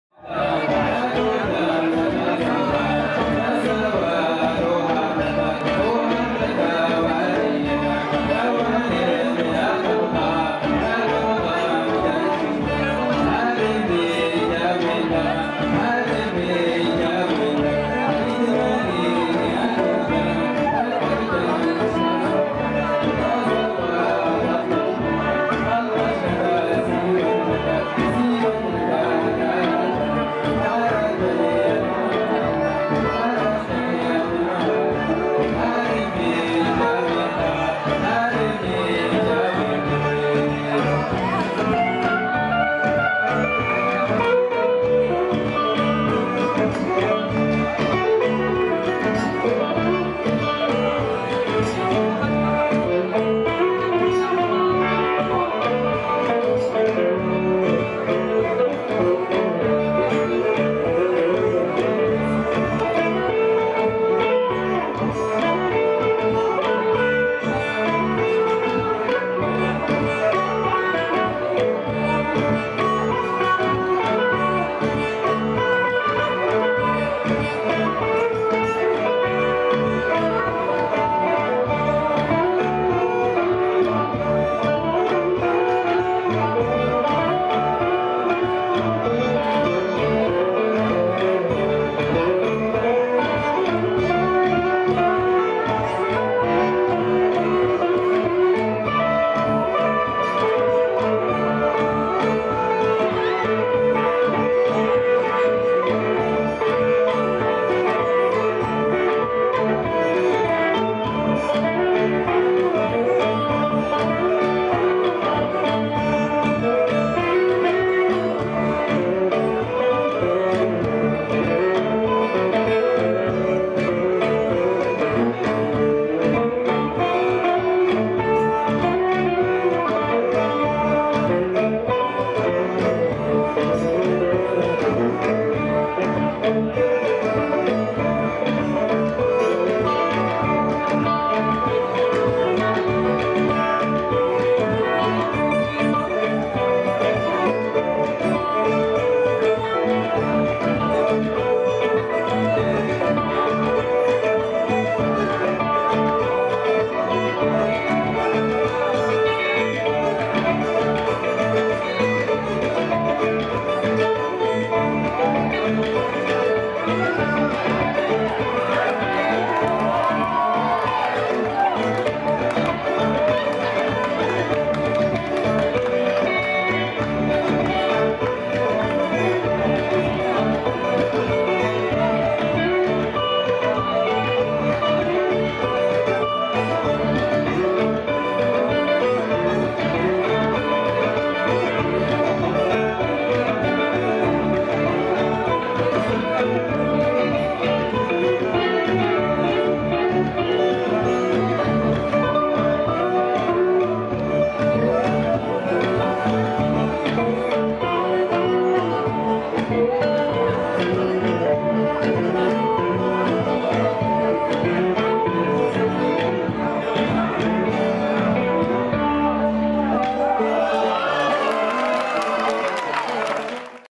Chet Azawad live in Nairobi
The Pan African Pentatonic Project
Sounds of the Sahel flowing with the Nile
​An artistic challenge based around two world-famous musicians: desert blues man Alhousseini Anivolla from Niger and Girum Mezmer, jazz guitarist from Ethiopia,